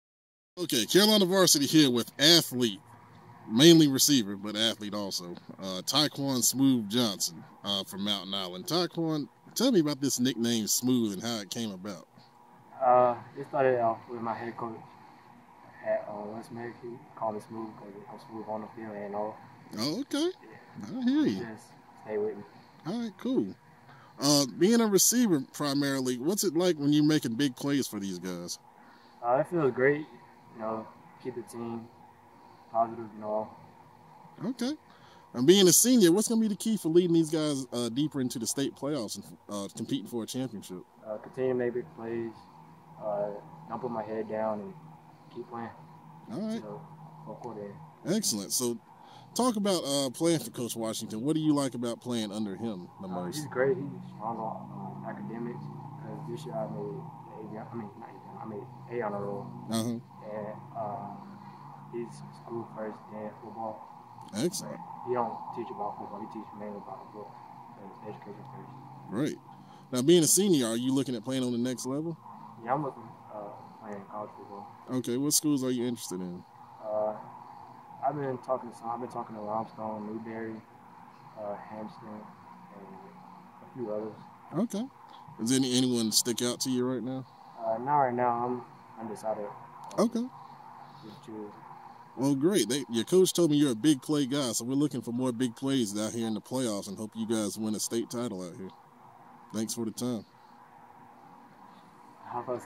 Carolina Varsity Interview - ATH Tikwan "Smooth" Johnson - Mountain Island Charter
Meeting With The Mountain Island Charter Raptors November 2016